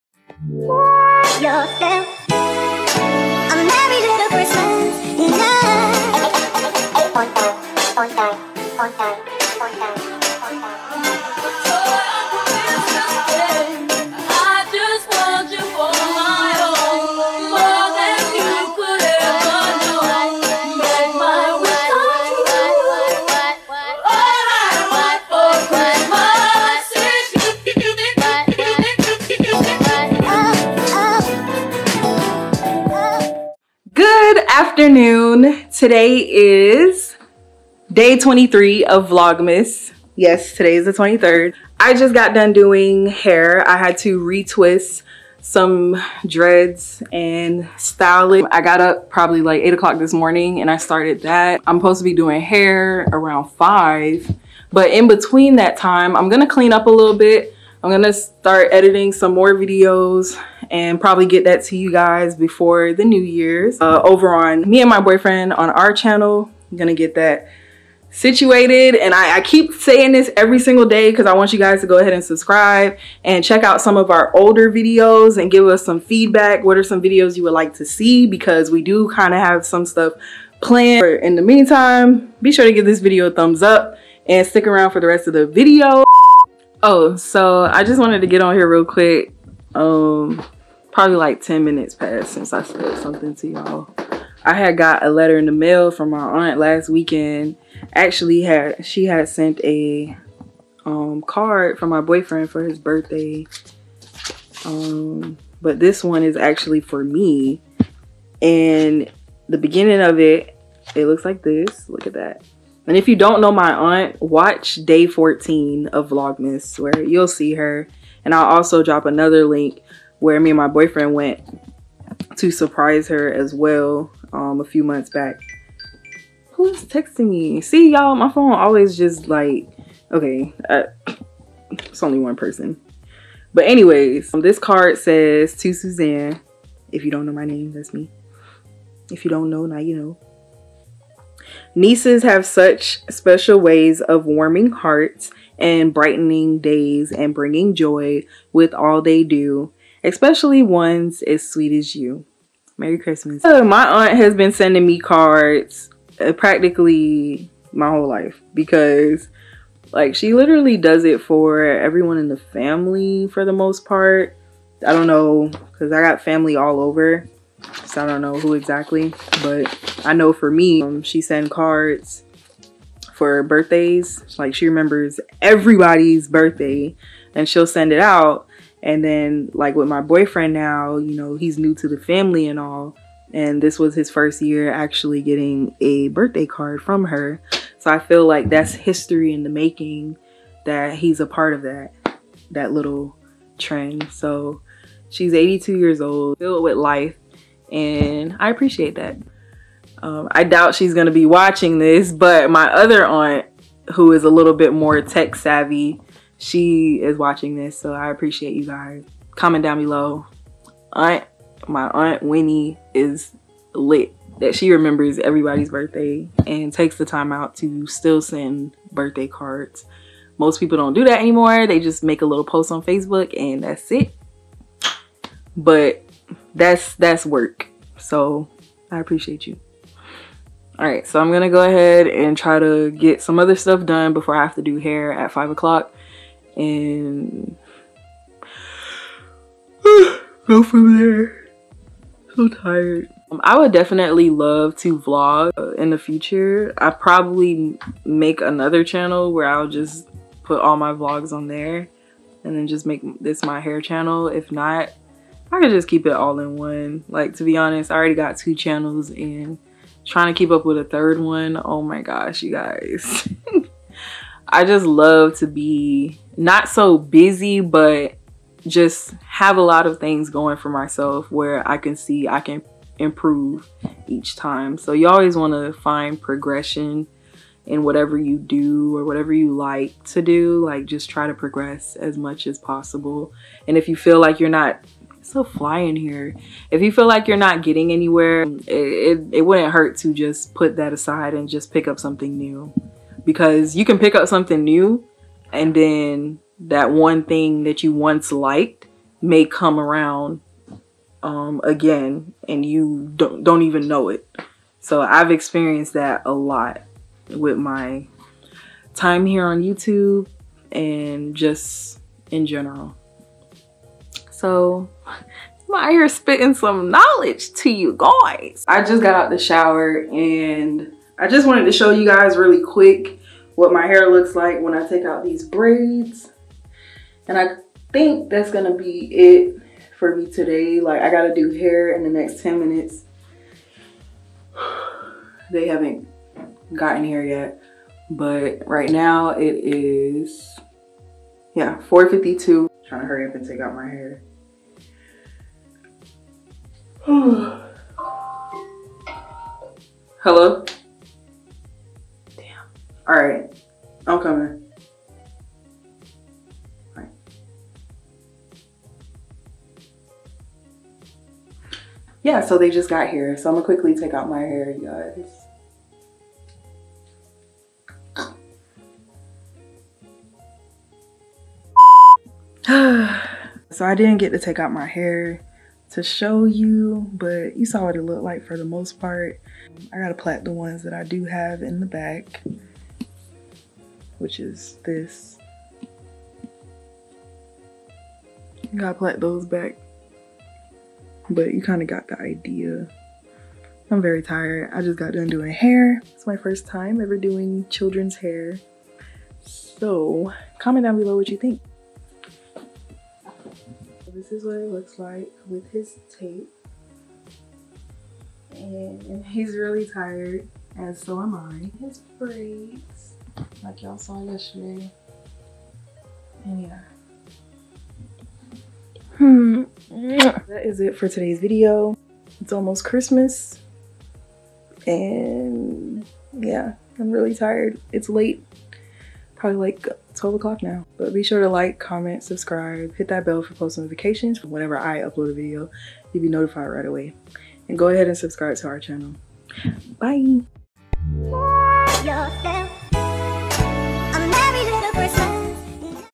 WORK DAY + UPDATE- 🎄VLogmas Day 23
DAY 23
In this video I am winding down for the holidays, I'm asking you guys to comment some video ideas for my couples channel because we have something planned soon !
Also, giving you insight on my day when it comes to hair styling.

If you have been following this vlogmas, I have mentioned several times that any extra activities will be presented on my couples  channel.

HERE ARE SOME VIDEOS YOU MAY LIKE⬇️

When my boyfriend and I went to surprise my aunt.